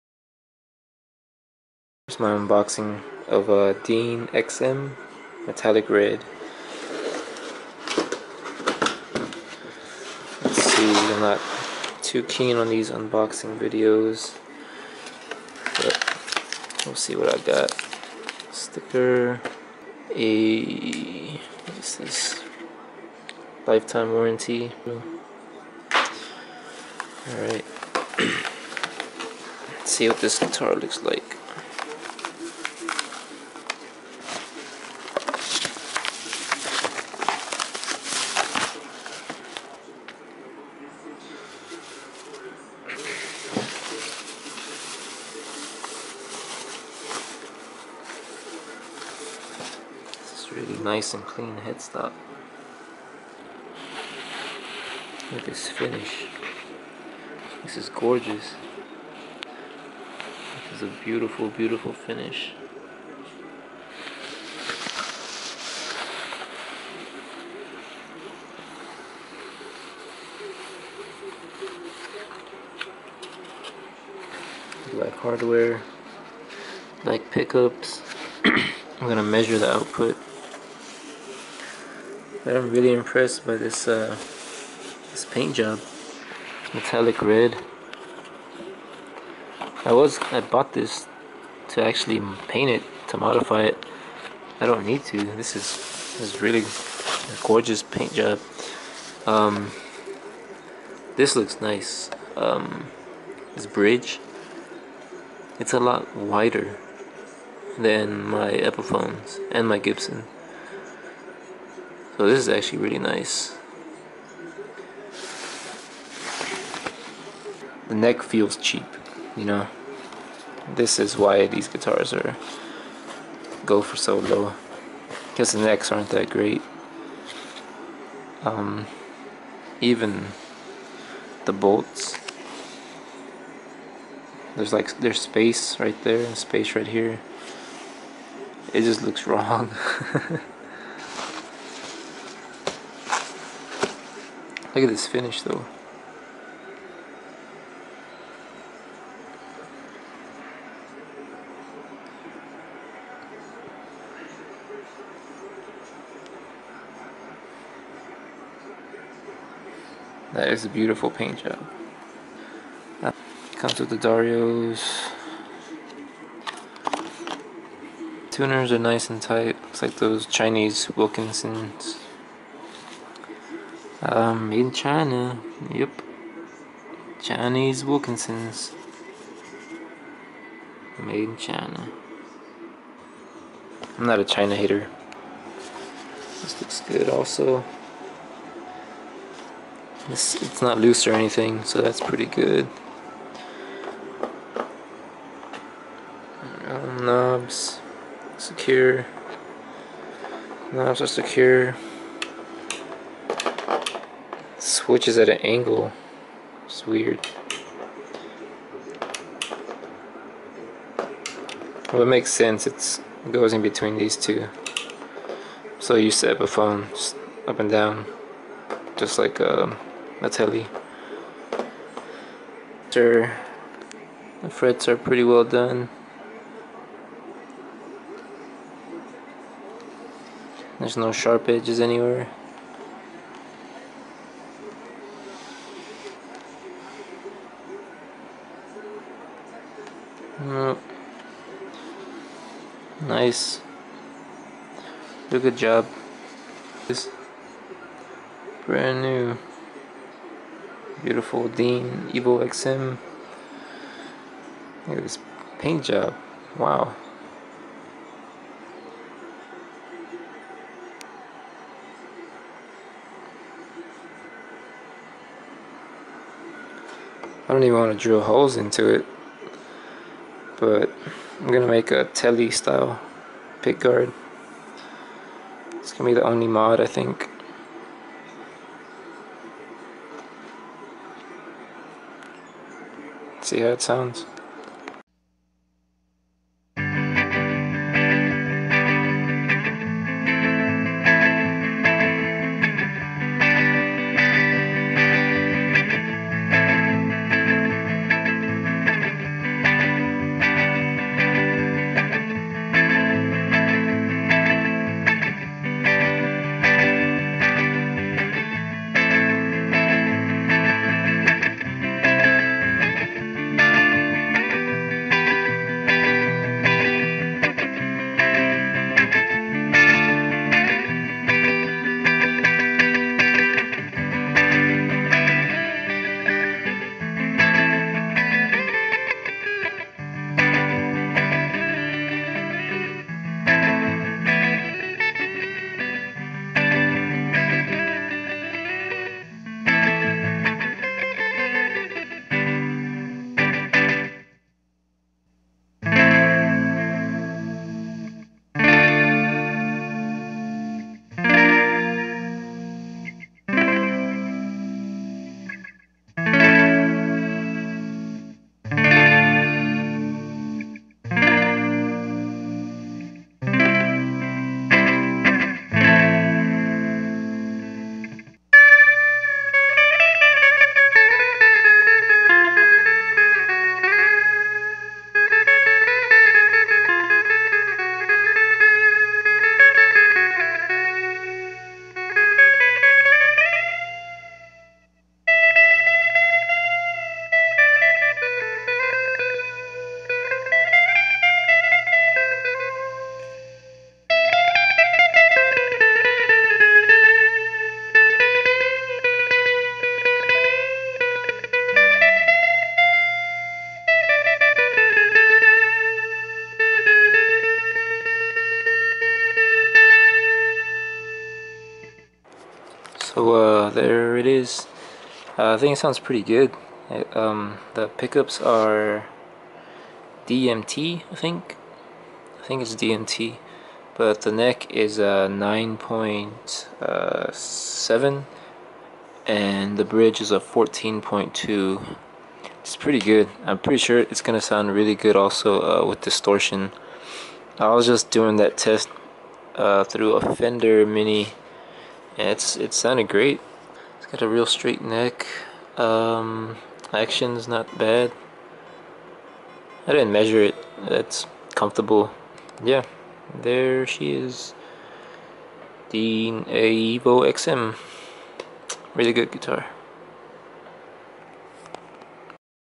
Dean Evo XM Electric Guitar Unboxing and Demo
Got a new Dean Evo XM electric guitar for dirt cheap and was pleasantly suprised by not only an amazing paint finish but it sounds pretty nice as well. It has a tele meets les paul shape to it which I like, instead of it just being a straight les paul copy. Anyways this is the unboxing and a short demo. Quick tidbits I left out, its 24.75 inch scale and has a rosewood fretboard. Also it weighs 9 lbs.